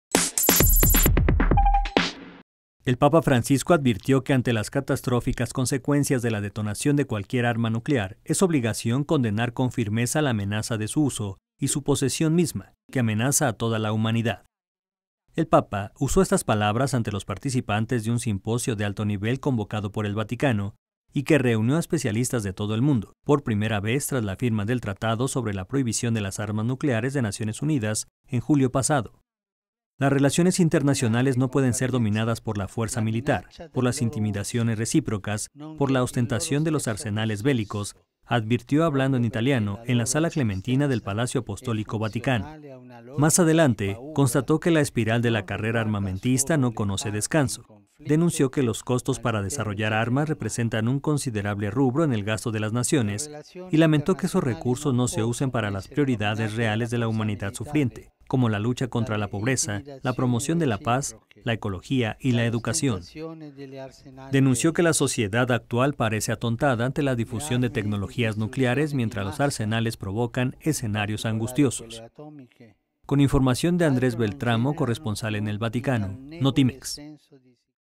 Papa Francisco condena uso y posesión de armas nucleares
El Papa Francisco advirtió  que, ante las “catastróficas consecuencias” de la detonación de cualquier arma nuclear, es obligación condenar “con firmeza la amenaza de su uso” y su posesión misma, que amenaza a toda la humanidad.